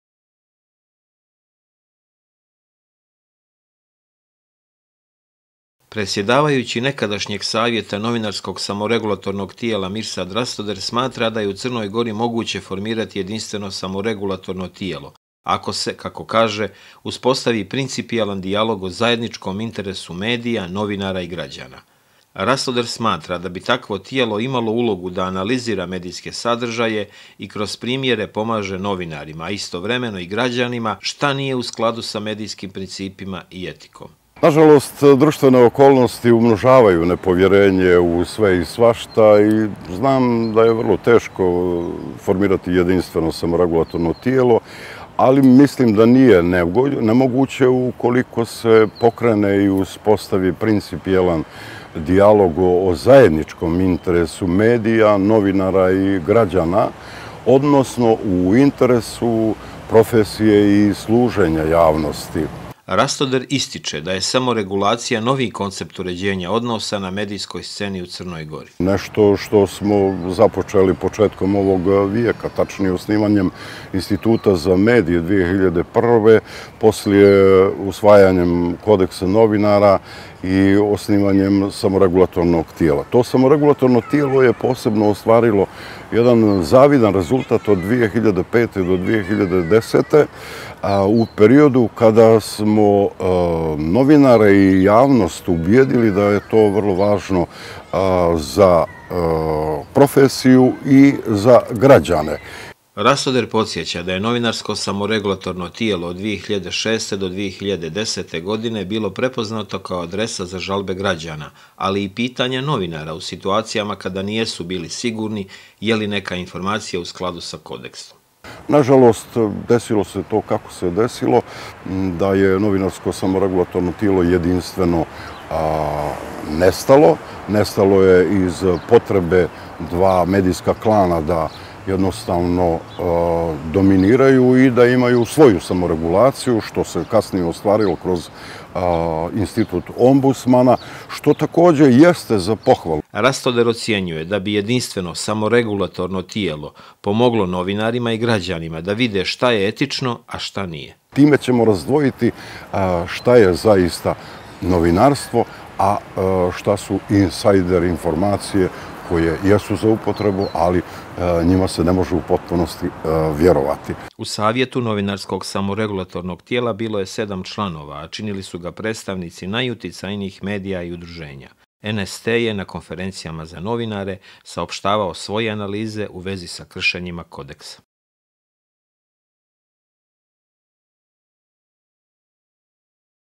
novinar Mirsad Rastoder
Mirsad Rastoder o radu nekadašnjeg Novinarskog samoregulatornog tijela